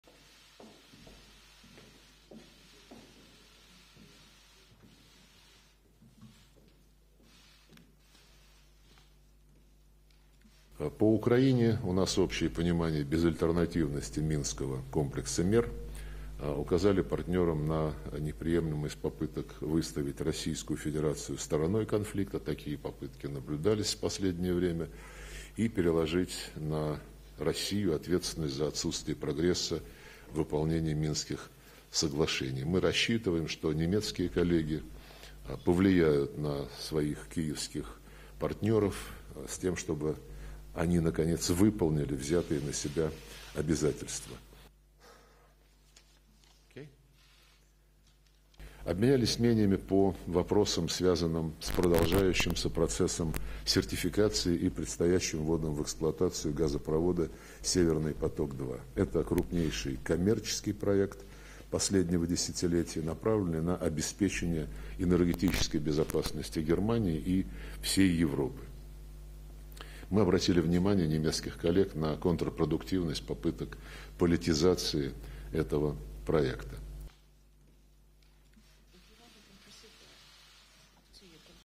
Russian, German FMs discuss Ukraine, Nord Stream 2
(18 Jan 2022) Russian Foreign Minister Sergey Lavrov and his German counterpart Annalena Baerbock discussed on Tuesday the situation in Ukraine and the Nord Stream 2 pipeline.
Speaking at the news briefing after the meeting in Moscow, Lavrov noted that it was unacceptable to "present Russia as part of the conflict" and to "shift the responsibility for absence of progress in implementing Minsk agreements to Russia".
Baerbock arrived in the Russian capital after a visit to Ukraine, with the relationships between the two countries sure to be the main topic of the conversation between the two diplomats.
Speaking Monday on a visit to Kyiv, Baerbock had warned of a "high price"  for any further escalation - and emphasized the need to continue negotiations.